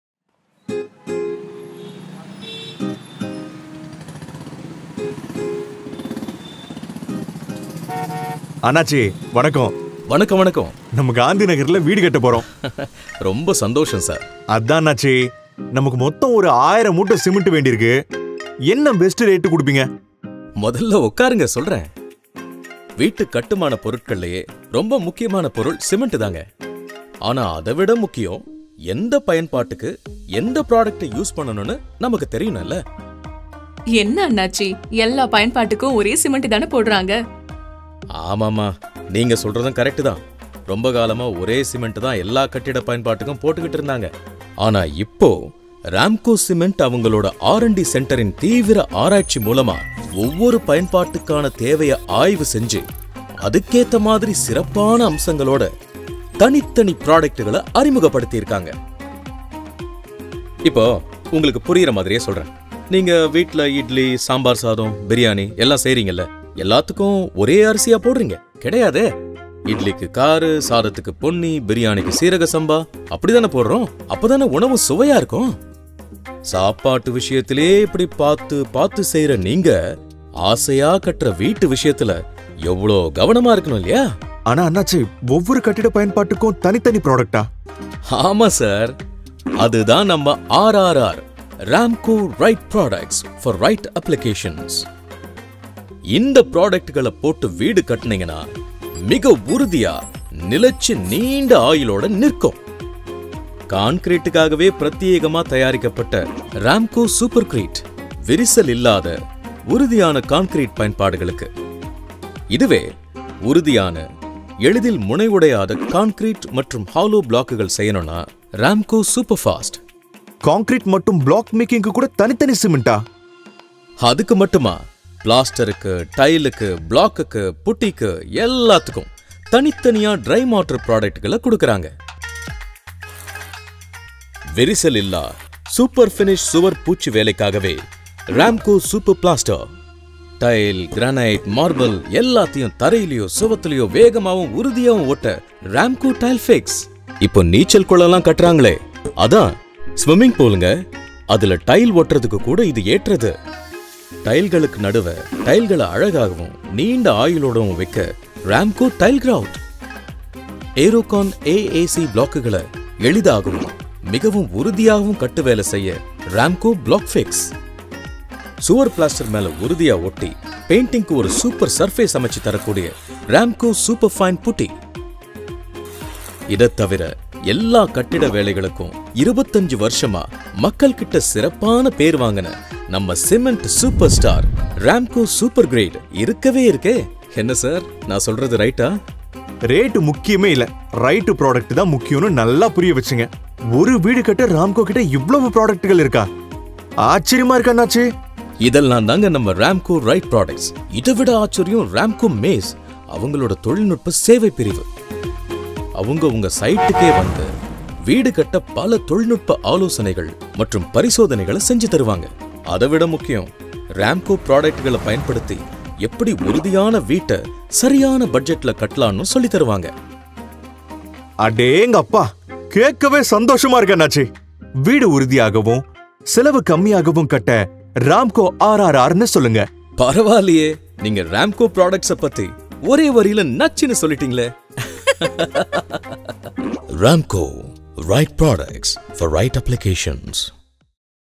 RRR - Tamil - Ramco Right Products for Right Applications
Building own home is a cherished dream for many of us. Do check out Ramco Cements' "Right Products for Right Applications" for everything your home requires.